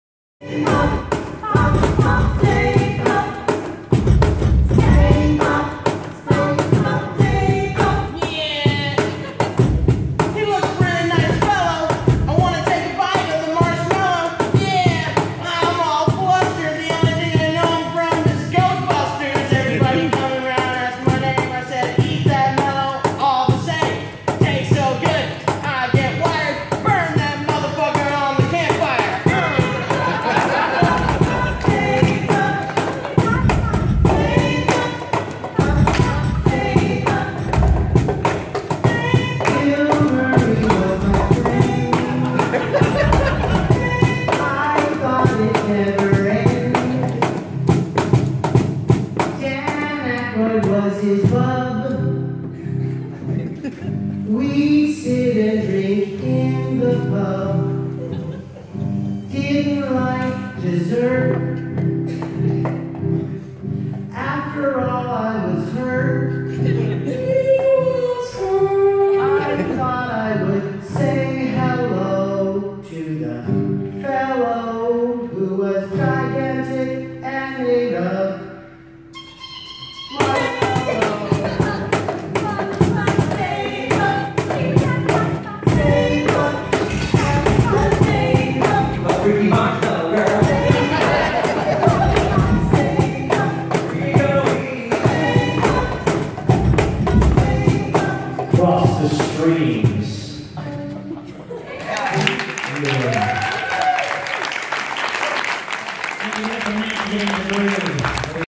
Nerd Core Stay Puft Insta Sing by Rock Comic-Con panel at New York Comic-Con
Based on a suggestion from the audience, these Nerd Core performers made up a song about the Stay Puffed Marshmallow Man on the spot. Unfortunately I missed the beginning of the song, but you get the idea.

Kirby Krackle, H2Awesome!, Daenerys & the Targaryens, My Little Dark Corner, Music Video Time Machine